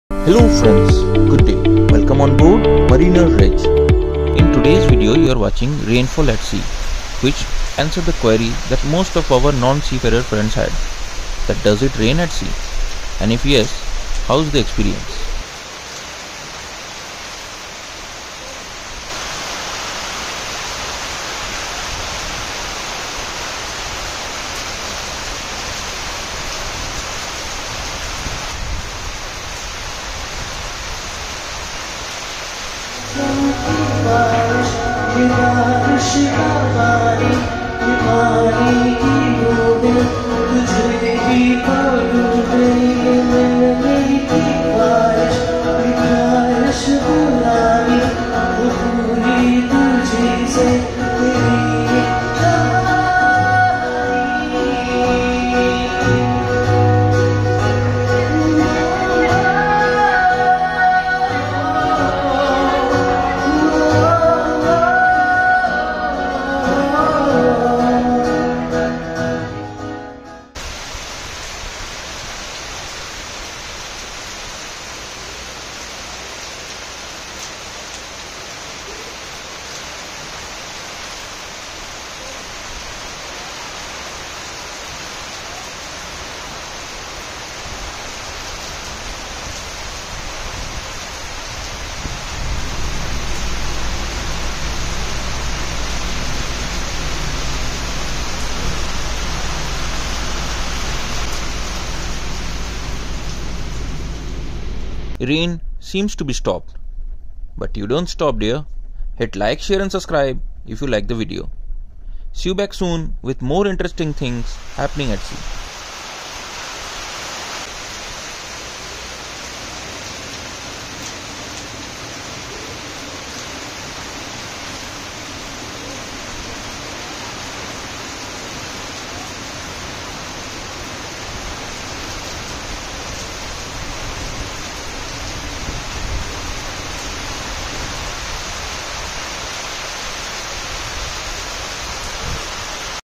Rain at Sea
Rainfall at Sea.

Many people have a query to know about Rain at Sea and How is the experience.
This video shows Rain at Sea during hours of darkness and Daylight.